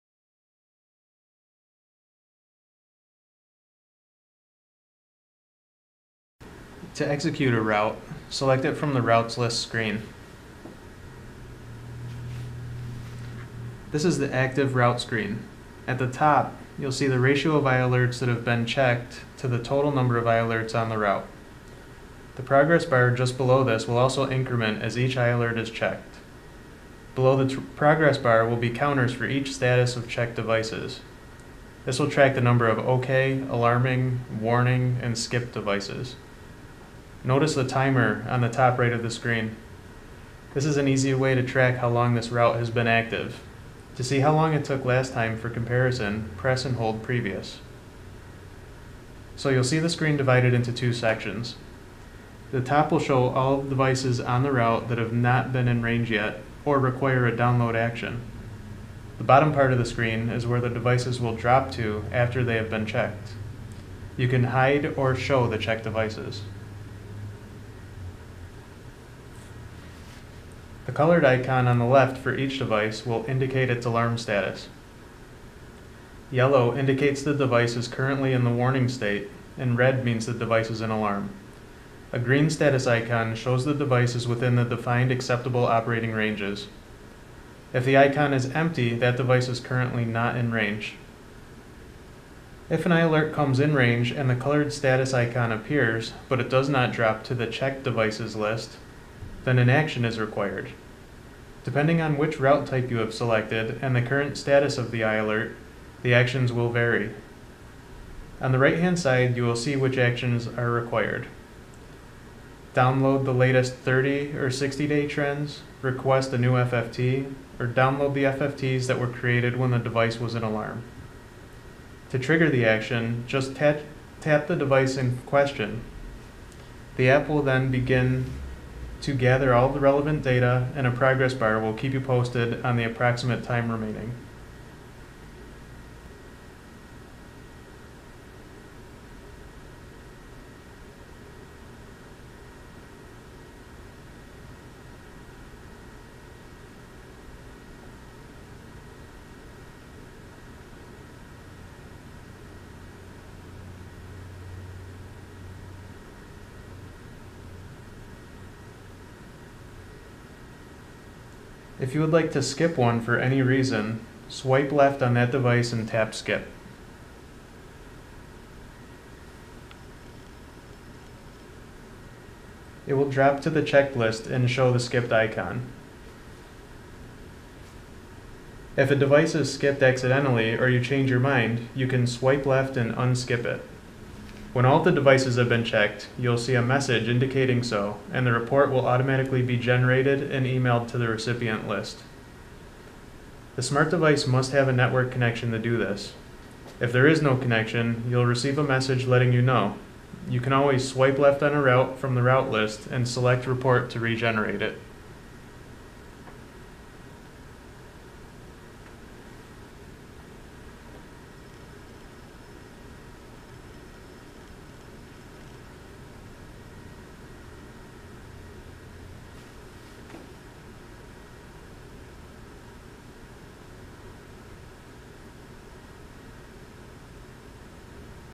i-ALERT App - Executing a Route
Learn how to execute a route in the i-ALERT2 app.